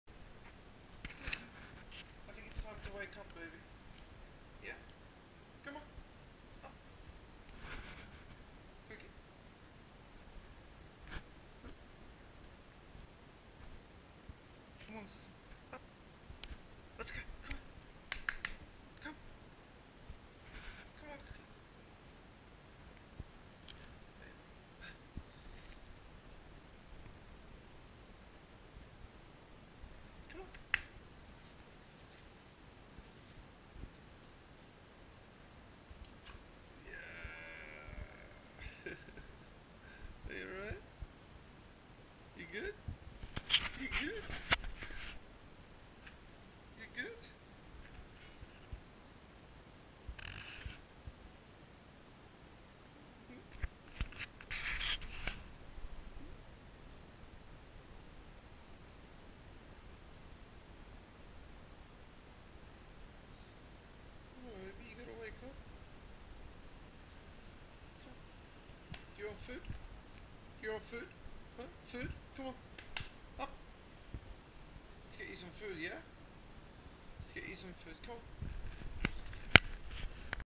When she doesn't feel like going for a ride.. (No Edit)
Cookie, won't wake up today.. to go for a ride.